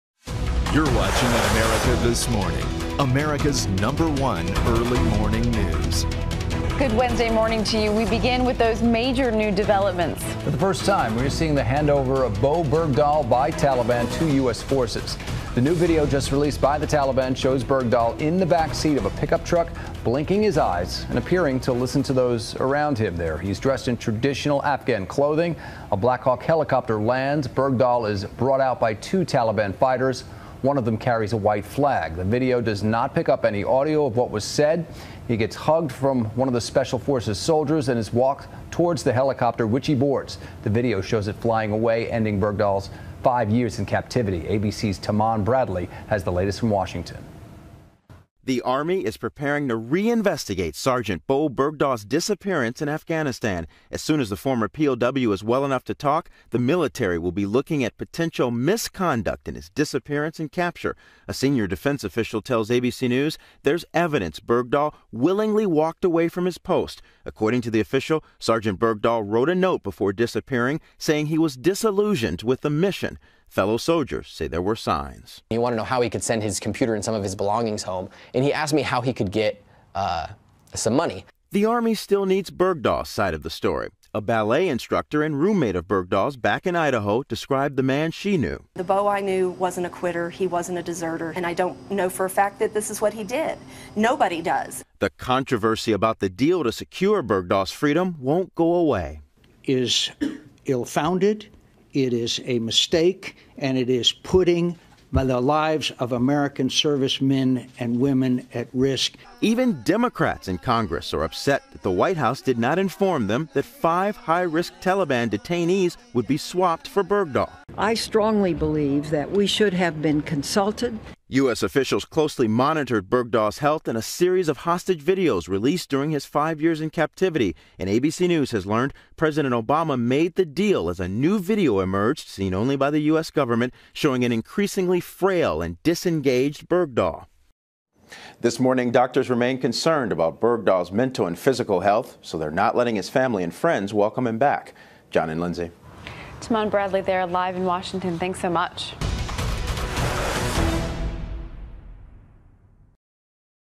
New Video Shows Release of Army Sergeant Bowe Bergdah
ABC News' Tahman Bradley has the latest on the Taliban video of Bergdahl's handover in Afghanistan.

*MORE: